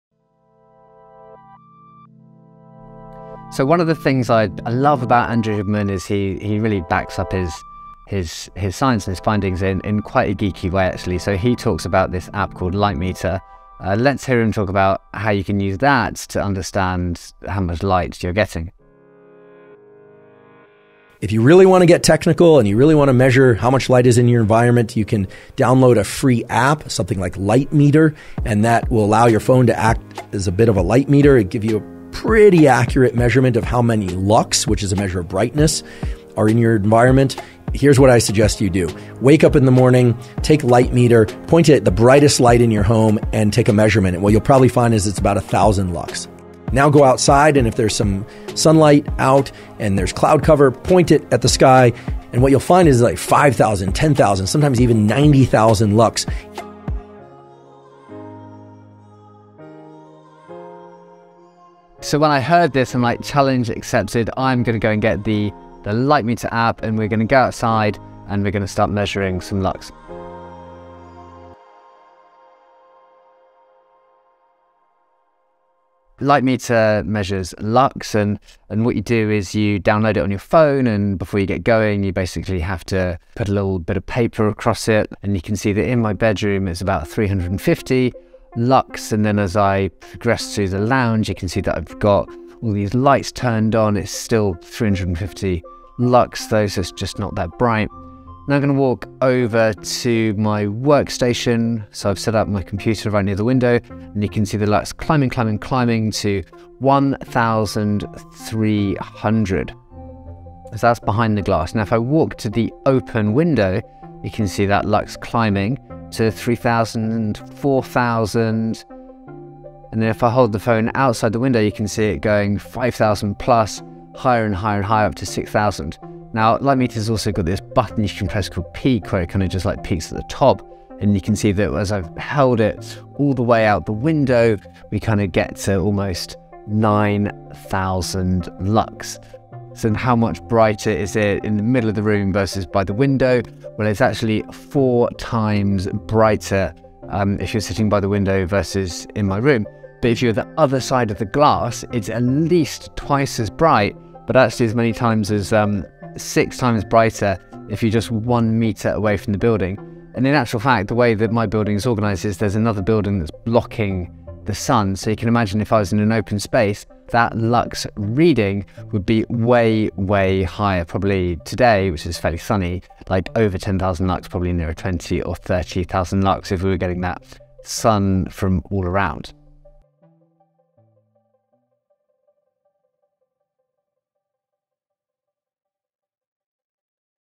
Huberman Test! Using Light Meter to Measure Indoor vs. Outdoor Light Intensity
Andrew Huberman is the director of the Huberman Lab, where he and his team study the neural circuits that underlie visual perception and behaviour. Dr. Huberman is known for his work on the effects of light and dark exposure on brain function and physiology. In this short video, we use the Light Meter app to assess light intensity indoors vs. outdoors, measured in Lux.

Lux is commonly used to describe the amount of light that is needed for various applications, such as reading, working, or outdoor activities. It is also used to measure the illuminance of artificial lighting systems, such as those used in homes, offices, and public spaces.

Earthy30.com